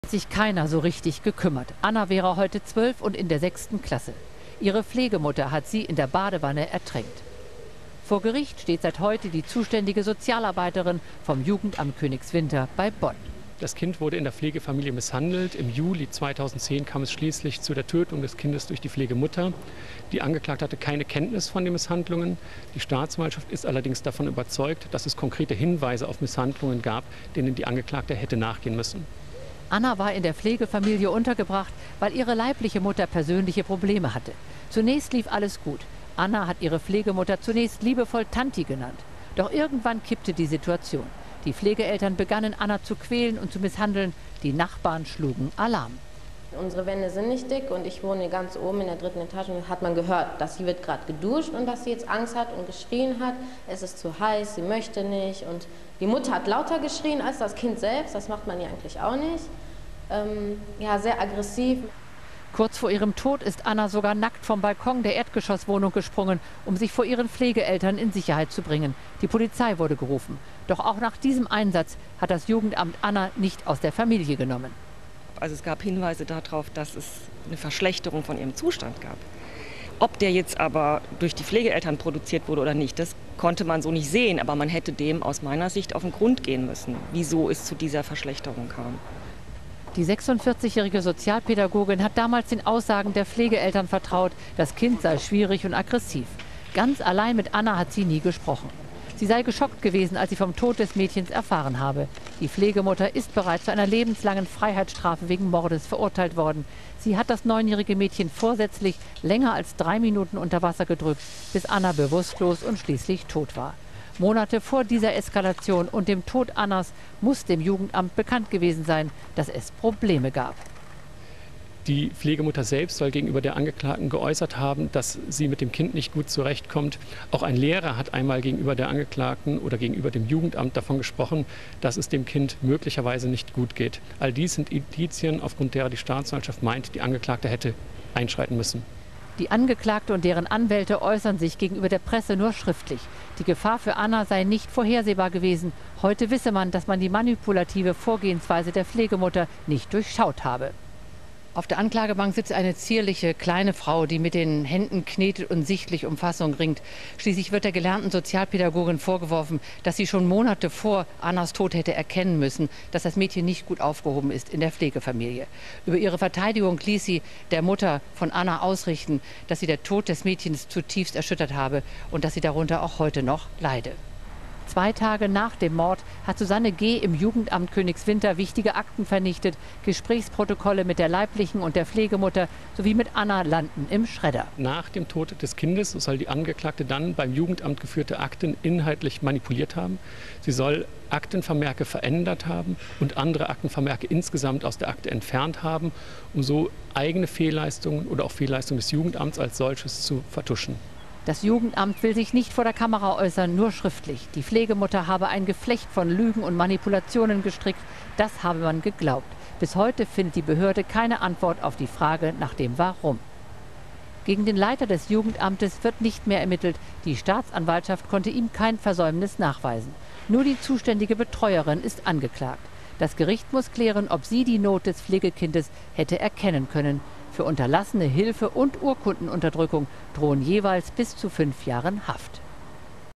Fall Anna - Der Tod kam vom Jugendamt ZDF 25.09.2013
Prozess gegen Jugendamtsmitarbeiterin
Hätte der gewaltsame Tod des neunjährigen Pflegekindes Anna aus Bad Honnef verhindert werden können? Am 22. Juli 2010 war das Mädchen nach jahrelangen Misshandlungen von ihrer Pflegemutter in der Badewanne ertränkt worden. Die Pflegeeltern wurden bereits verurteilt.

Jetzt steht auch die ehemalige Mitarbeiterin des Jugendamtes Königswinter, die für das Pflegekind Anna zuständig war, vor Gericht. Der 45-jährigen Sozialpädagogin werden fahrlässige Körperverletzung im Amt durch Unterlassen in 26 Fällen sowie Urkundenunterdrückung und Verwahrungsbruch vorgeworfen. Trotz zahlreicher Warnhinweise habe sie das Kind nicht aus der Familie genommen. Den Befehl zur Aktenvernichtung erteilte der freigesprochene Jugendamtsleiter!